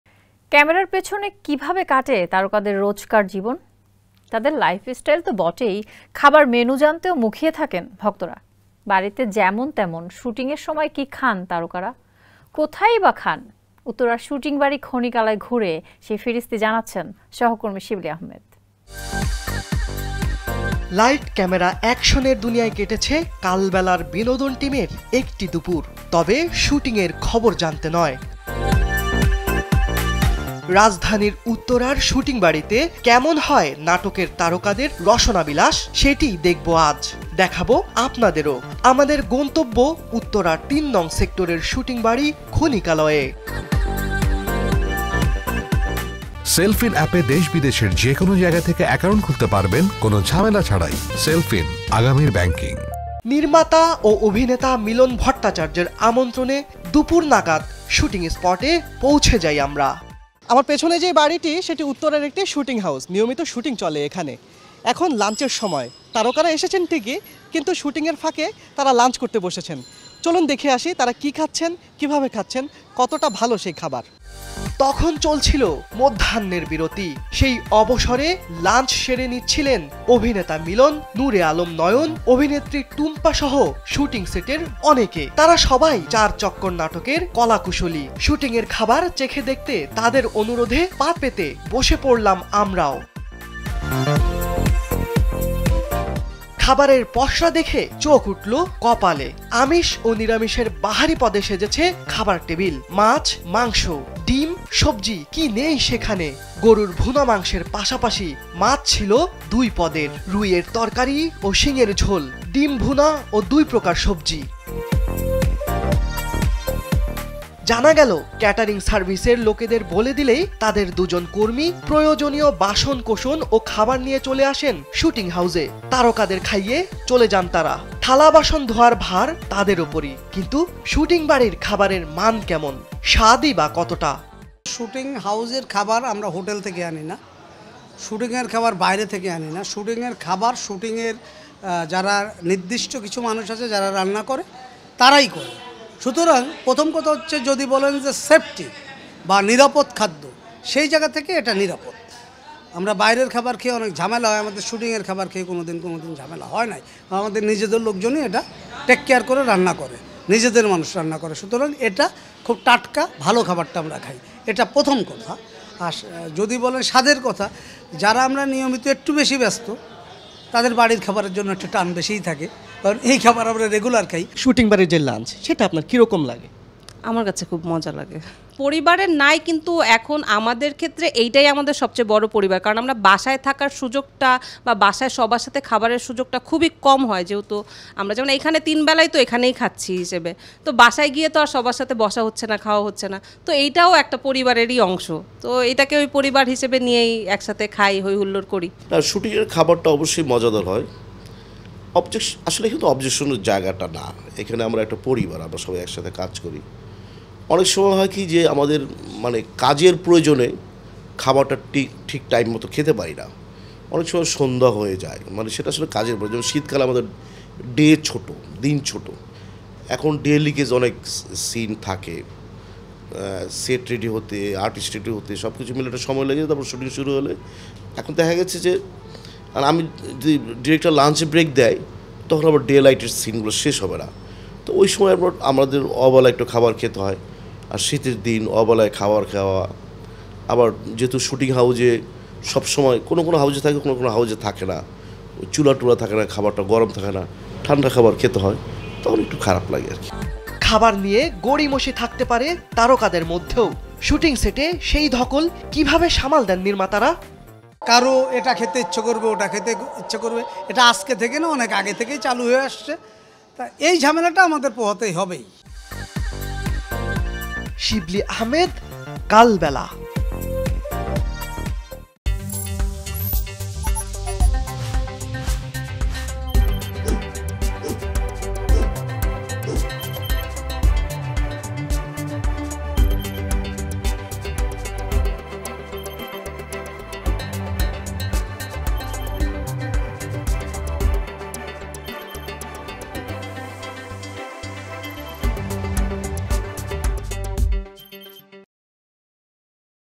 শুটিংয়ের দুপুরে কী খান তারকারা? | Shooting Time | BD Celebrity Lunch | Kalbela
লাইট ক্যামেরা অ্যাকশনের দুনিয়ায় কেটেছে কালবেলার বিনোদন টিমের একটি দুপুর। তবে শুটিংয়ের খবর জানতে নয়, রাজধানীর উত্তরার শুটিংবাড়িতে কেমন হয় নাটকের তারকাদের রসনাবিলাস, সেটিই দেখব আজ, দেখাবো আপনাদেরও। আমাদের গন্তব্য উত্তরার ৩ সং সেক্টরের শুটিংবাড়ি ক্ষণিকালয়ে। শুটিংয়ের দুপুরে কী খান তারকারা? | Shooting Time | BD Celebrity Lunch | Kalbela